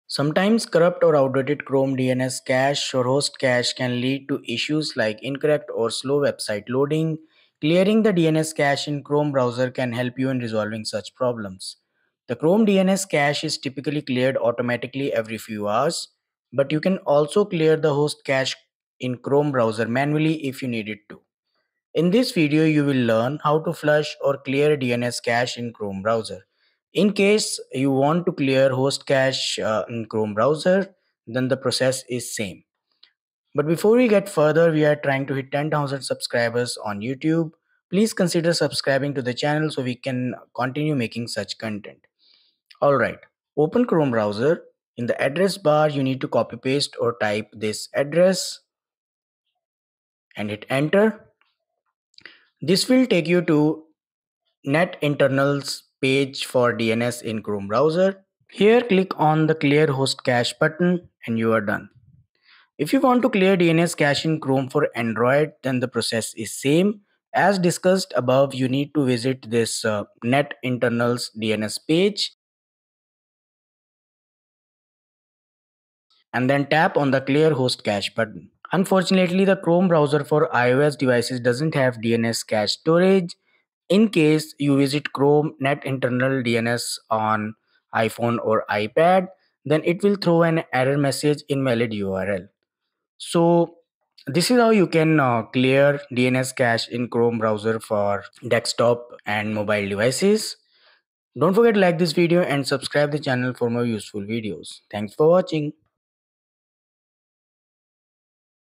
How to Clear Host Cache in Chrome (DNS Cache) | Chrome Net Internals
In this video, you will learn how to clear host cache in Chrome (DNS Cache) and Chrome Net Internals.

In case, you're looking for a way how to flush DNS cache in Chrome

FOLLOW@GEEKERMAG
FOLLOW@GEEKERMAG
FOLLOW@GEEKERMAG

RECOMMENDED DOWNLOADS